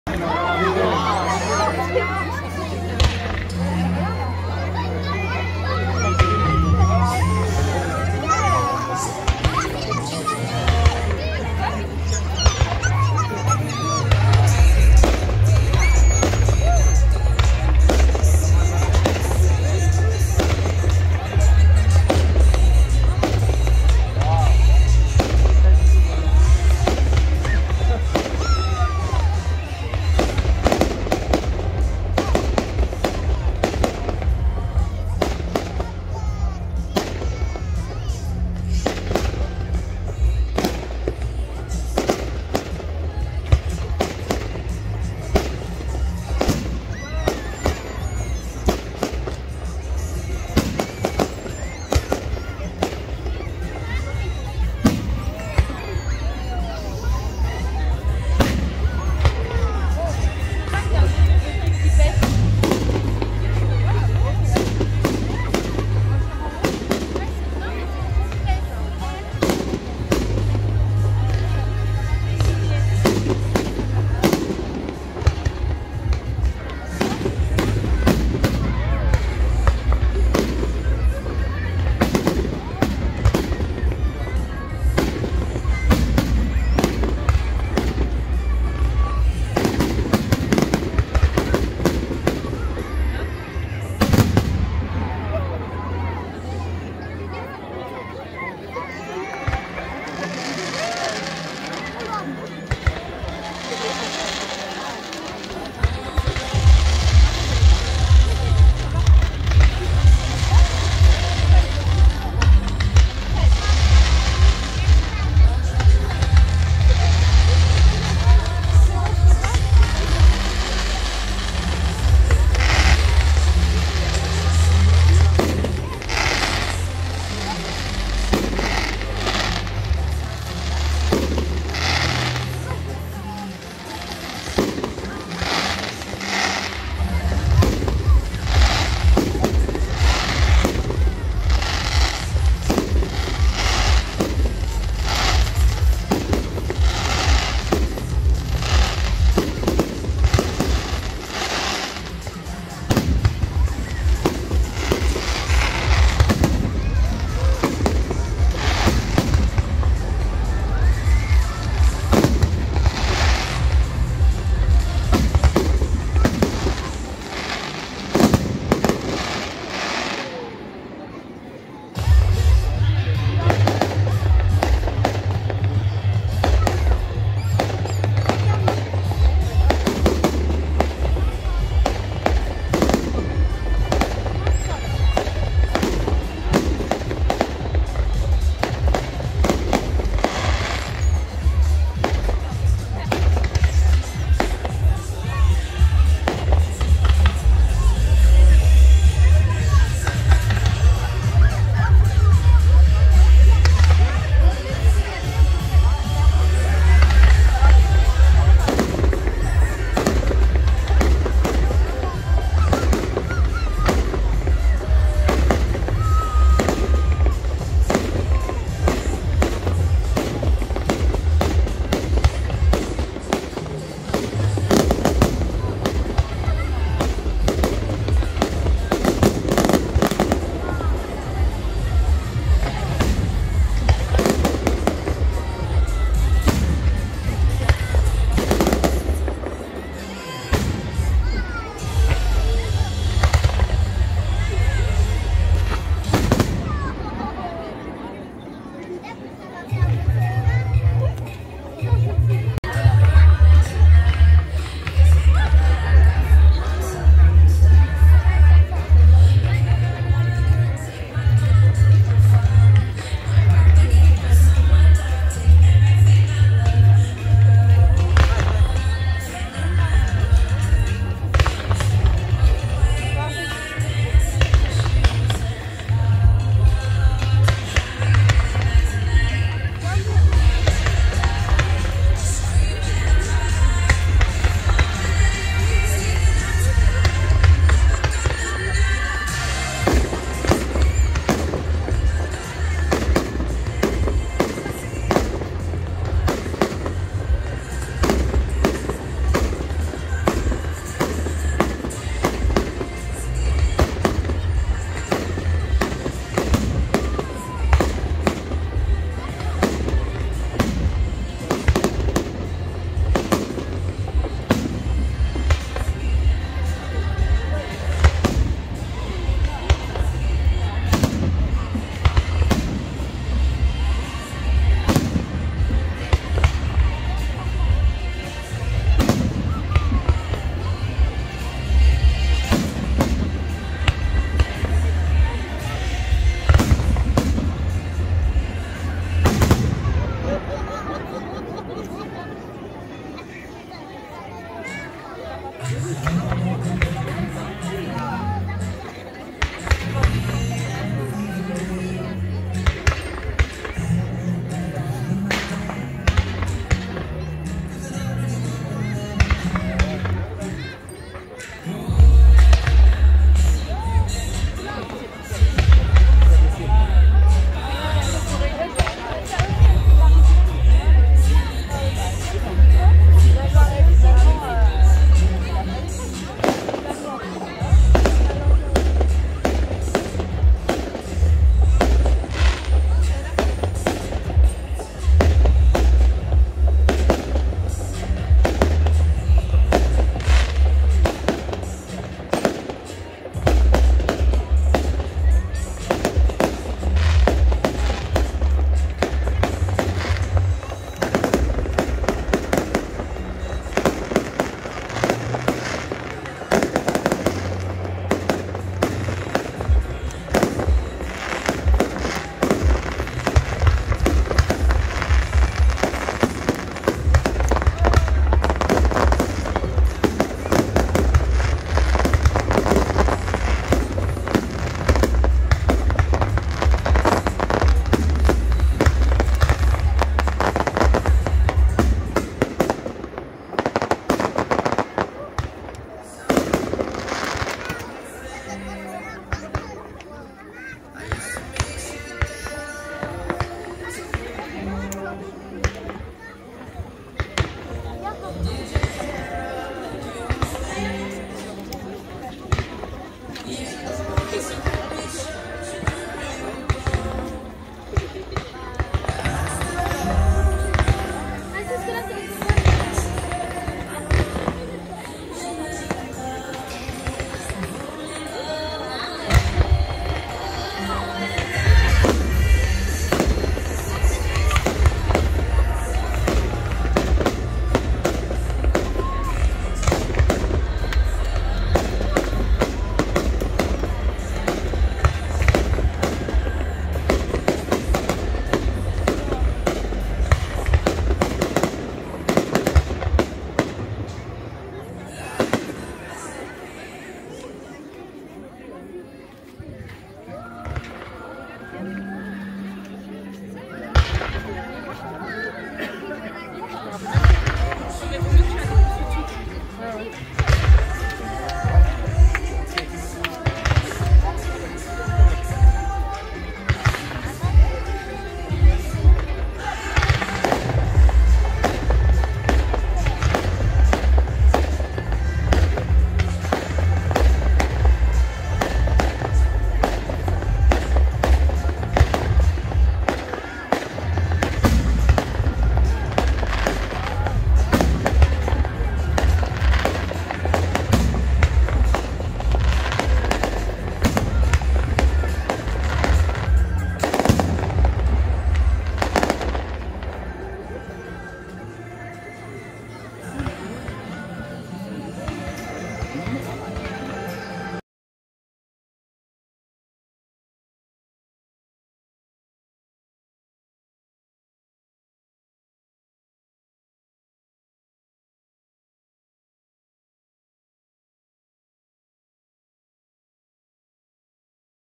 Feu d'artifice 2023 @ Chenove Côte-d'Or
Feu d'artifice is a fireworks 🎆 done in France 🇫🇷 in celebration 🍾 of Bastille day. French holiday.